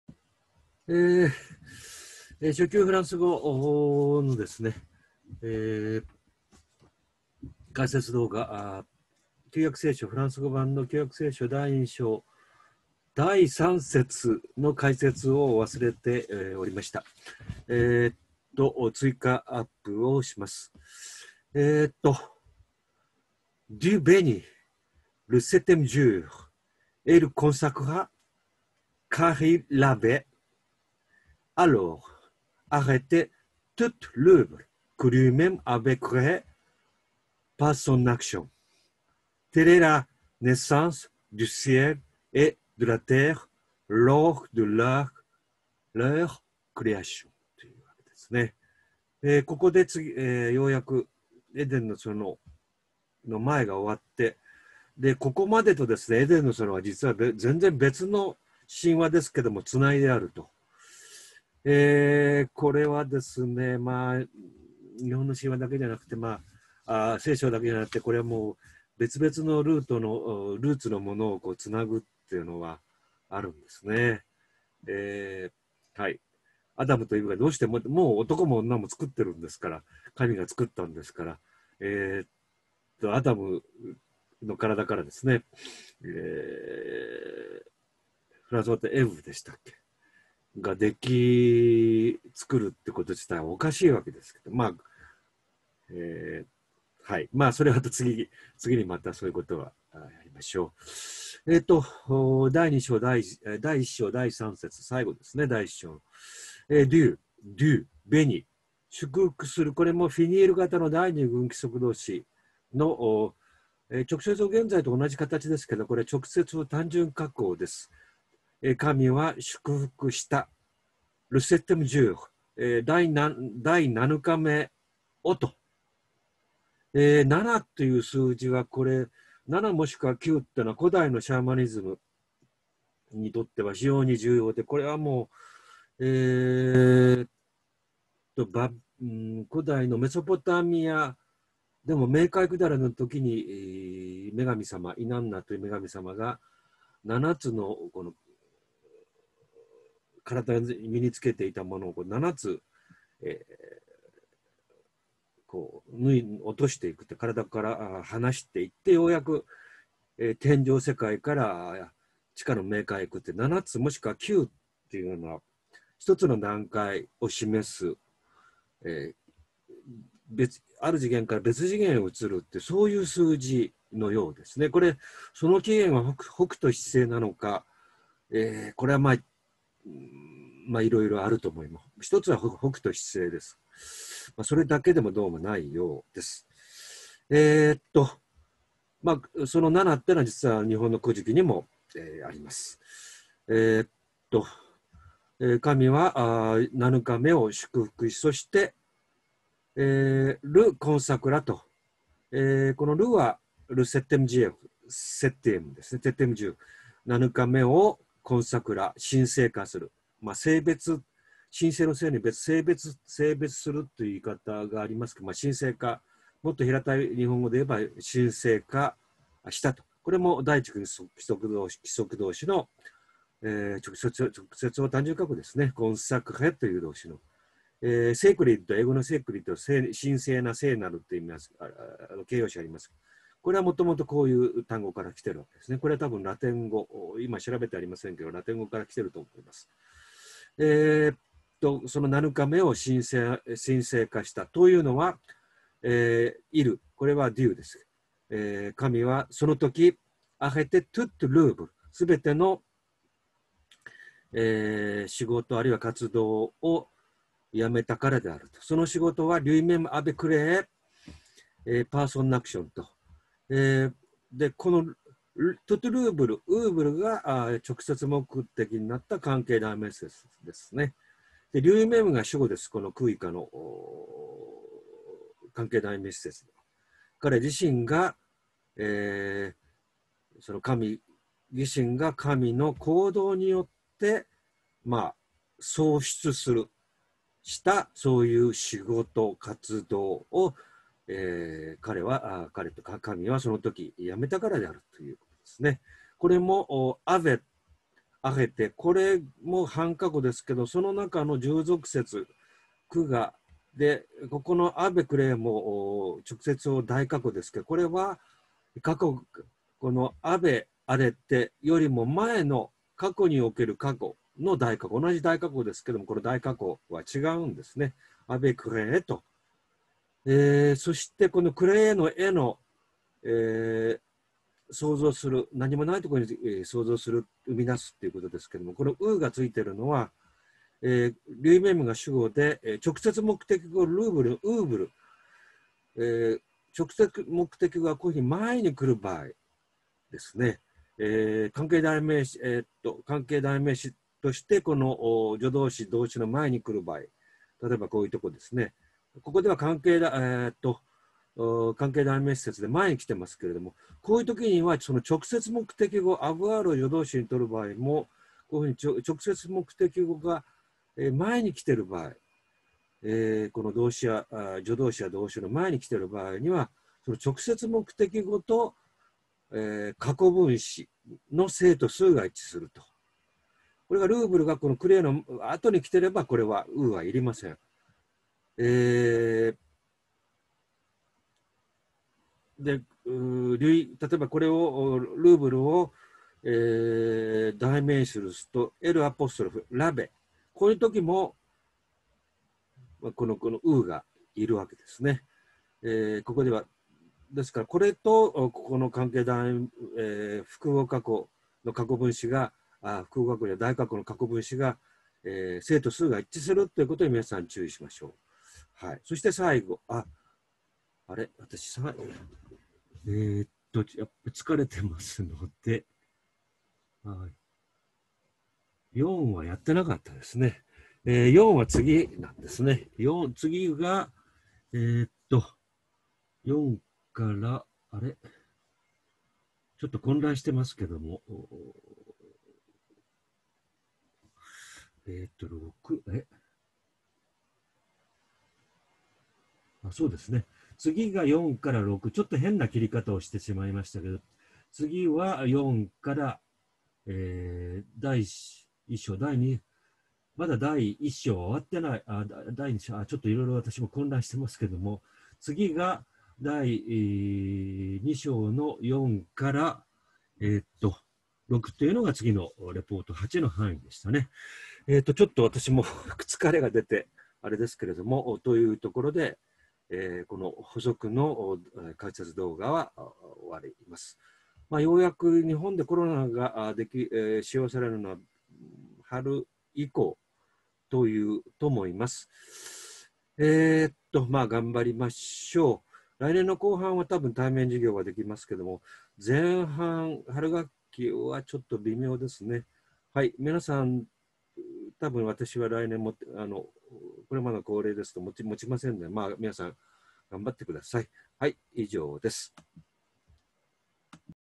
旧約聖書 レポート７解説２
２０２０年度秋学期
明治大学法学部初級フランス語１年９組
フランス語共同訳旧約聖書第２章第３節
TRADUCTION ŒCUMÉNIQUE DE LA BIBLE, Seconde Edition, Editions des Sociétés Bibliques, France, 1987.